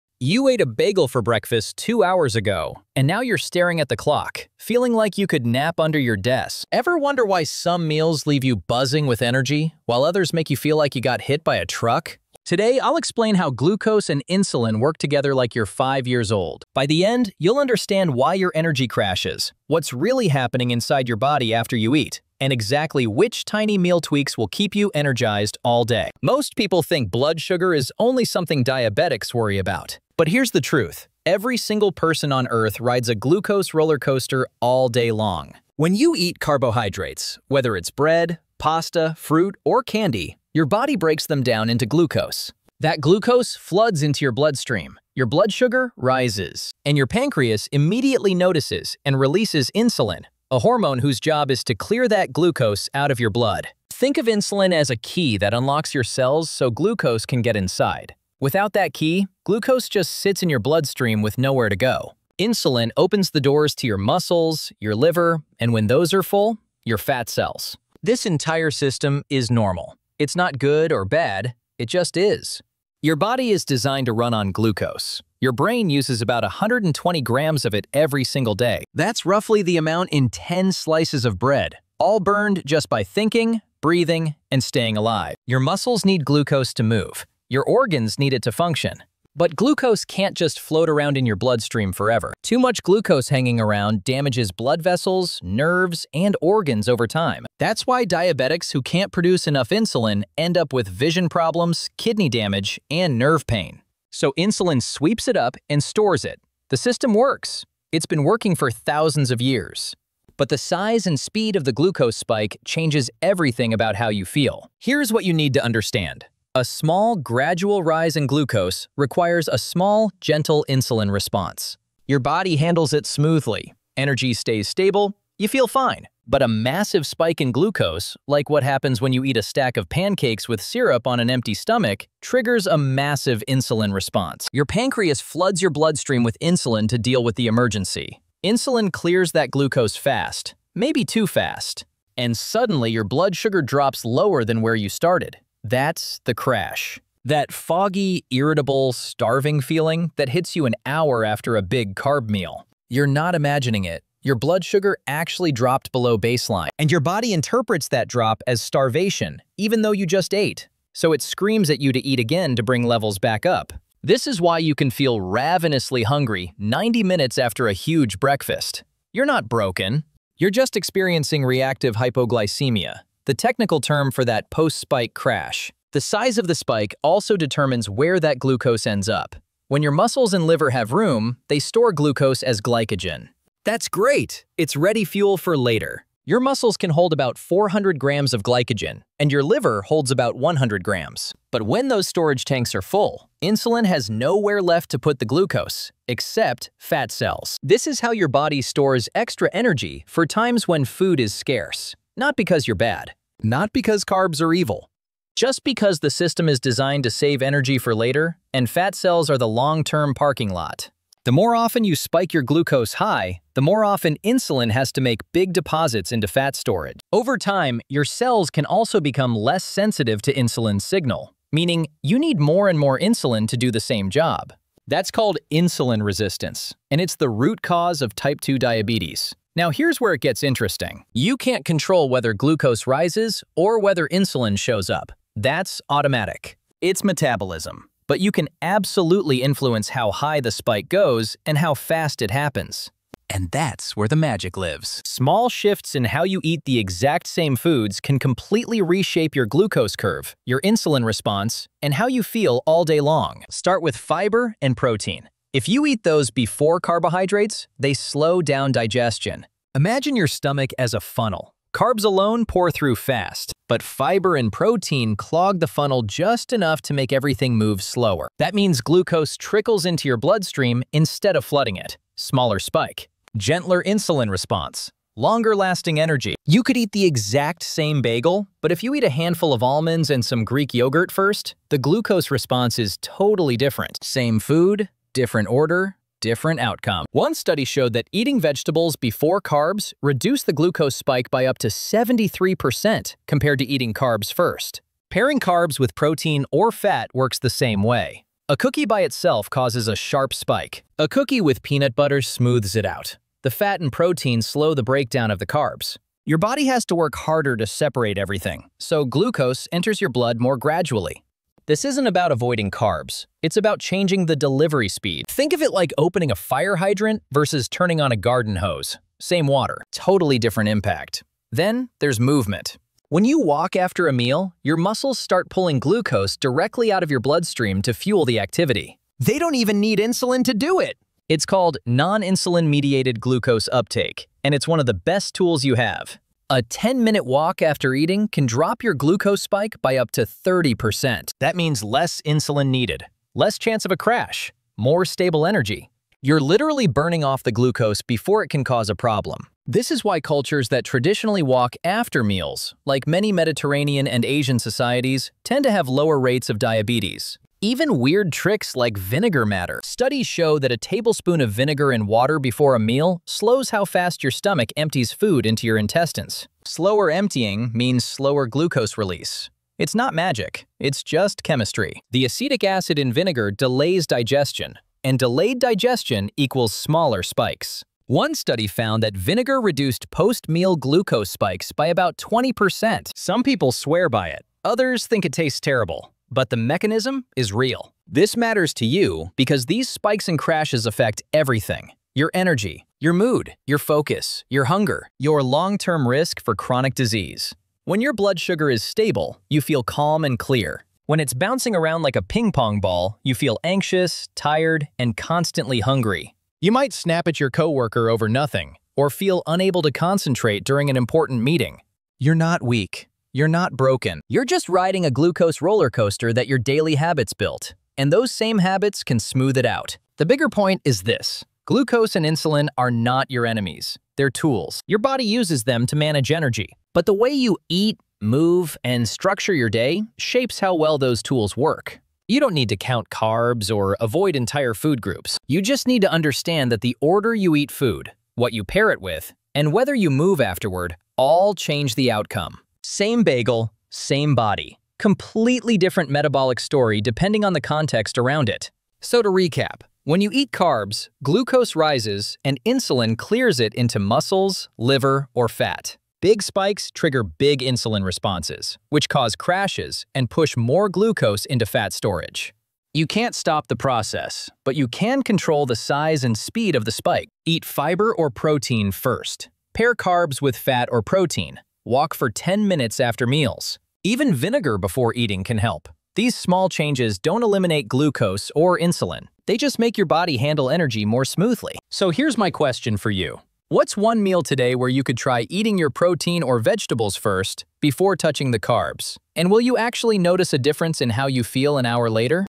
This Eating Order Trick Cuts Blood Sugar Spikes By 73%
Ever feel exhausted after breakfast? 🥯😴 That bagel crash isn't in your head—it's your blood sugar! In this video, I break down exactly how glucose and insulin work together, why you experience energy crashes, and the simple meal tweaks that'll keep you energized all day long.

Learn why eating protein BEFORE carbs can reduce glucose spikes by 73%, how a 10-minute walk after meals changes everything, and why your afternoon slump is totally preventable. This isn't about avoiding carbs—it's about eating smarter! 🧠⚡

Discover the science behind stable energy and say goodbye to that 3pm crash. Your metabolism will thank you! 💪